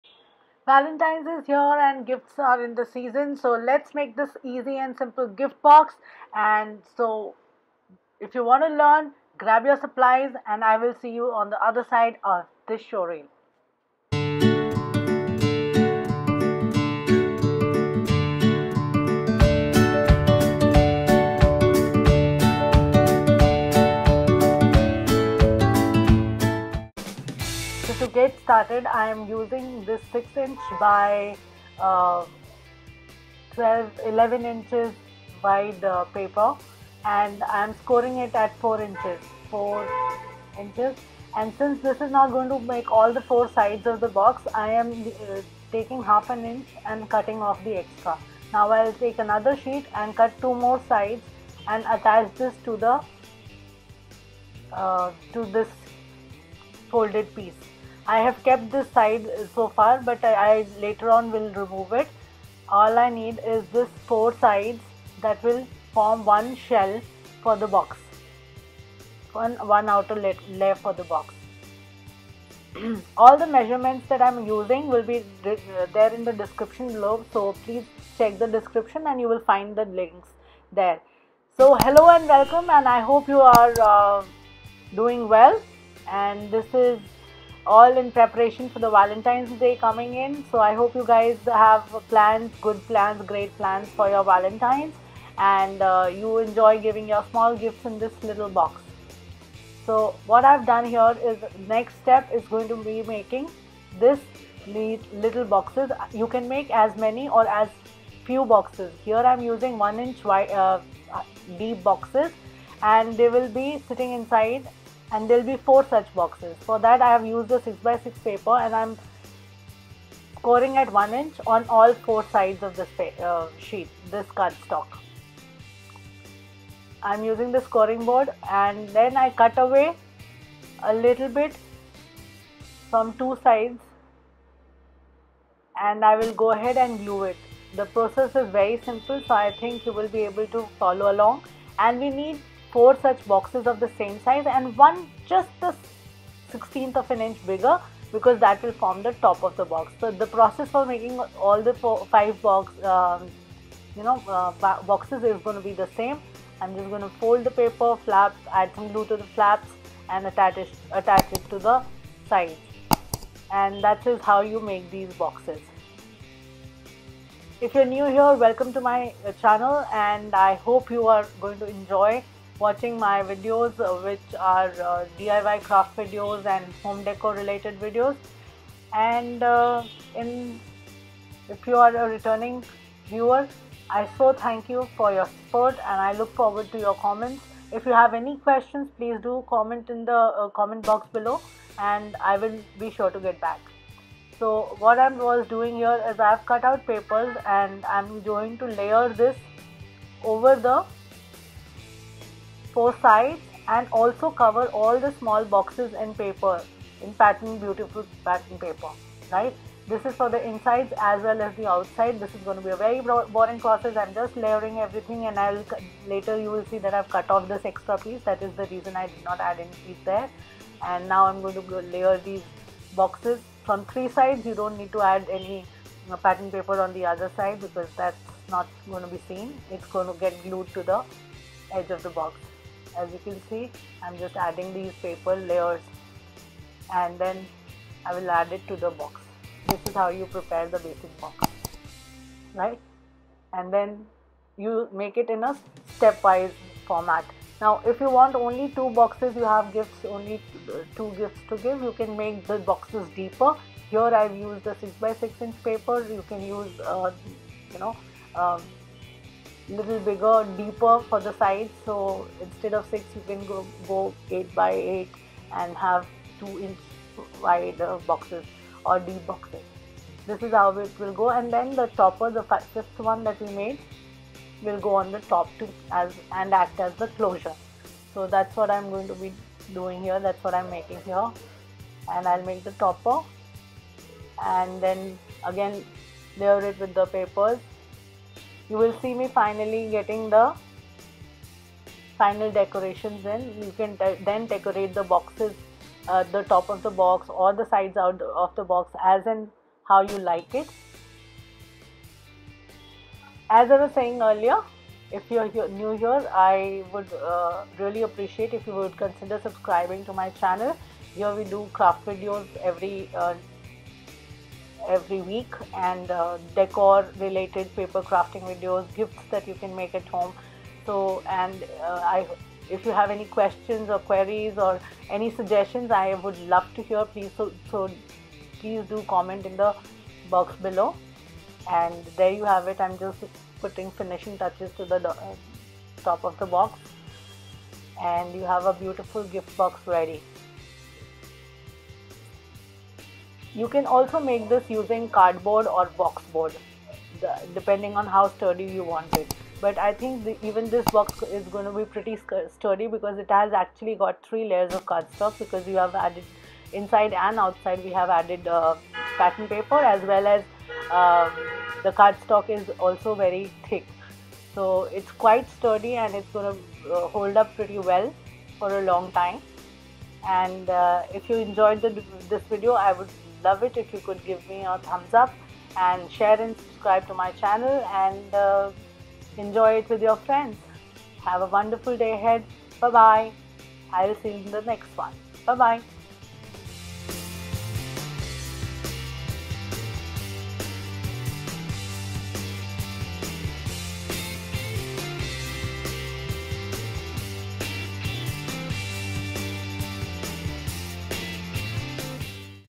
Layered Gift box DIY
hello Friends,
In this video we are making a Layered Gift box, that can hold up to 4 small gifts and chocolates and even photos, this is a very simple and easy to make project. It is not only beautiful but makes a great memorabilia holder.
for the outside box shell:-
Card stock size 6"x11" scoring at 4 ", 8", 8.5" cut away the excess
Card stock size 6"x11" scoring at 4 ", 8", cut away the extra piece
inside boxes:
cardstock size 6" scoring at 1" on all sides
top cover
cardstock size 6 1/16" scoring at 1" on all sides
cardstock Black
white glue
scrapbooking pattern paper
decorative pieces like flowers and leaves

Equipment used for shooting and editing:
Mobile phone: Redmi Note 8
Portable mobile holder arm
MacBook Air:
iMovies software